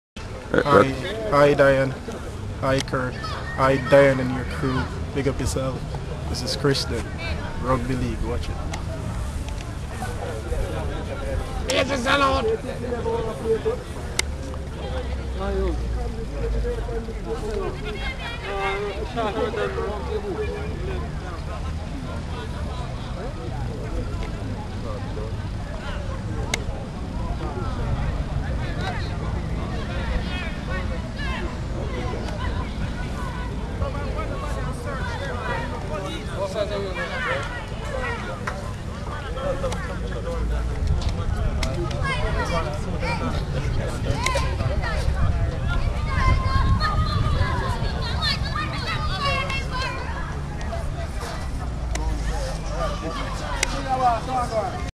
Christian Graysoion Jamaica Rugby League Shout Out
Christian gives a shout out to his freinds before reffing di big semi-finals Sharks vs Vuktures